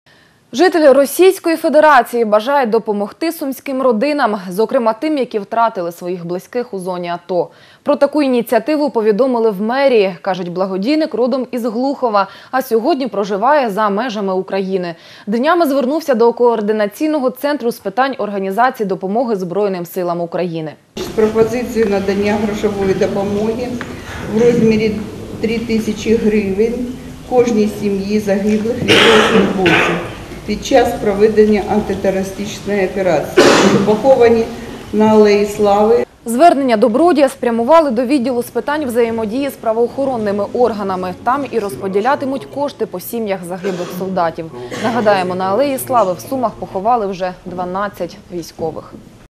Житель Російської Федерації бажає допомогти сумським родинам
Зокрема, тим, які втратили своїх близьких у зоні АТО. Про таку ініціативу повідомили в мерії.